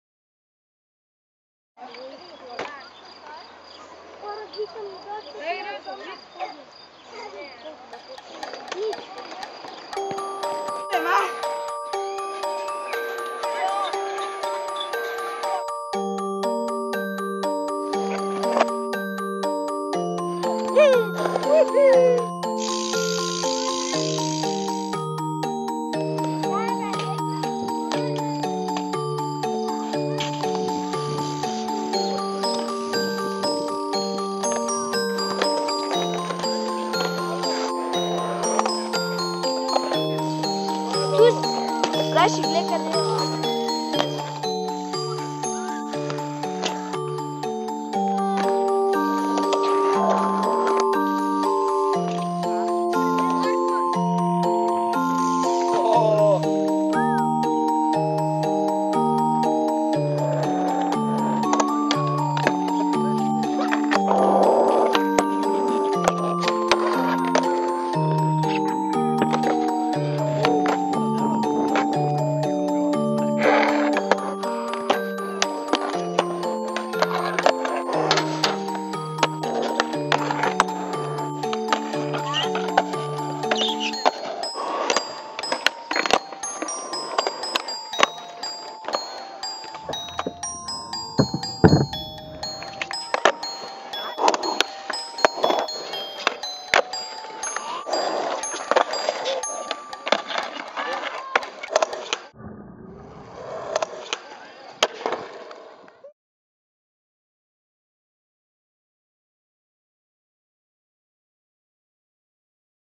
Planet Ljudski Vrt
some Skate & BMX footage from the oldest twon of little Slovenia
really cool place & people

Nik Zorec
Oskar Korošec
Miha Fištravec
Nik Matici
Benjamin Ziernicky
Rajko ("Bec")
Peter Štrafela